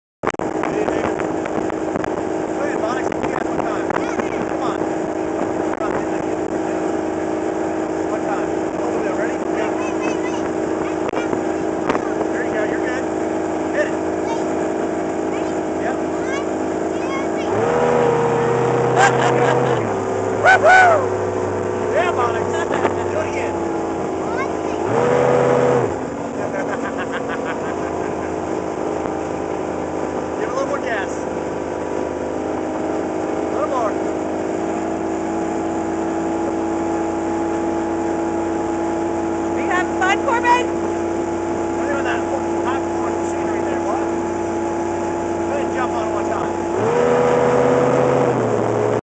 7 year old drives hydrostream V king
7 yr old son drives dad's v-king and likes it!